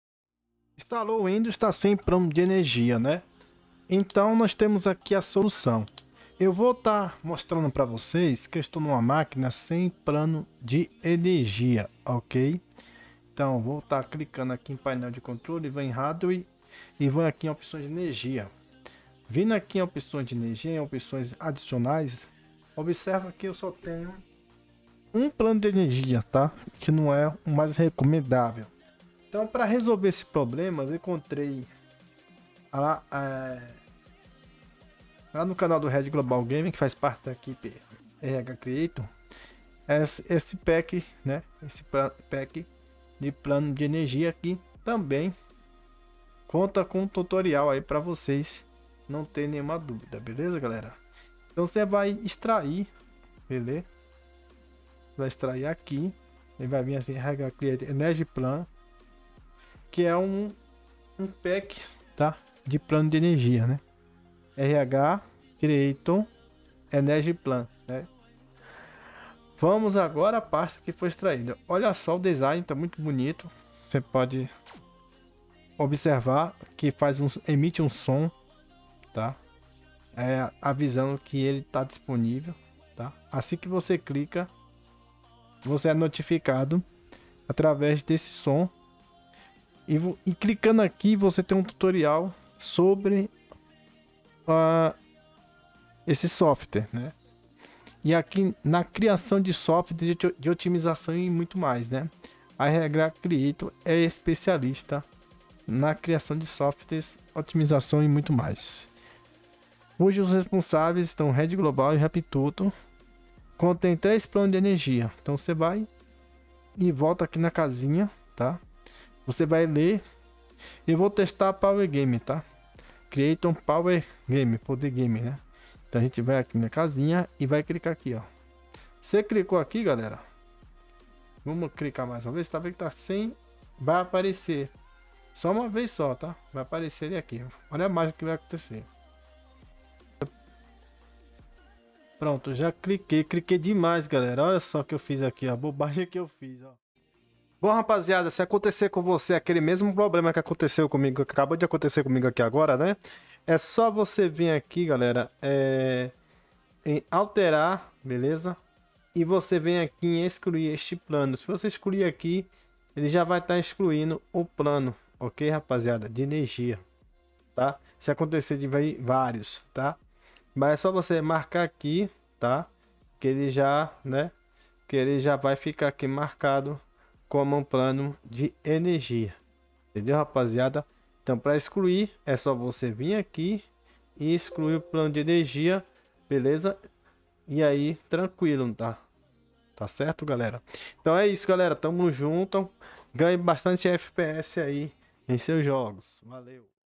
GANHE FPS EM SEUS JOGOS USANDO ESSE SOFTWARE DE PLANO DE ENERGIA!
CPU：Intel(R) Core(TM) i5-3340S CPU @ 2.80GHz
Placa-mãeTobefilledbyO.E.M._H61MXV/H61MXV-LE/H67MXV
Memória total：7.7 GB
Memória disponível:3.9 GB
Gráficos:Intel(R) HD Graphics
SSD:111GIGA
Parâmetros：CPU(2 núcleos) RAM(2048M) Ativo(1) Velocidade alta(1)
Resolução：1024x768 dpi 500 06)fps 60
CREDITO: